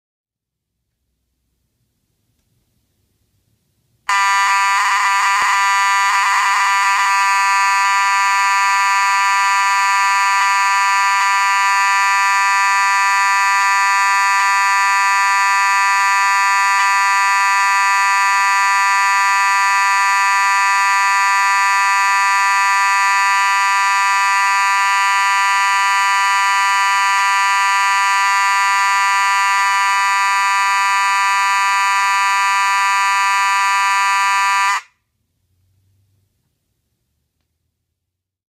Federal Signal Vibratone 450D Series D1 + V1971 Horn/Strobe
Test of a Federal Signal 450D with the Series D1 mechanism, a newer Faraday design incorporating a circuit board to generate the pulses to the electromagnet, along with a slanted V1971 strobe.